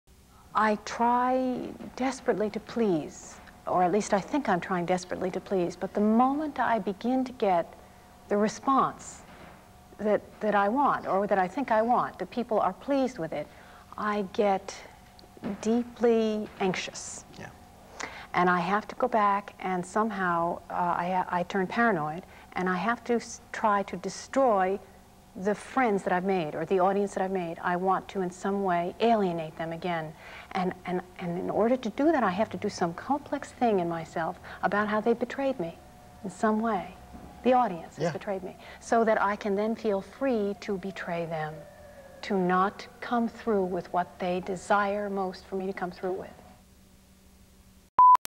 from the archives - Liz & Peter Sellars on BRT Belgian Radio & Television – 1986 [12.22.14]
In this video: Elizabeth LeCompte, Peter Sellars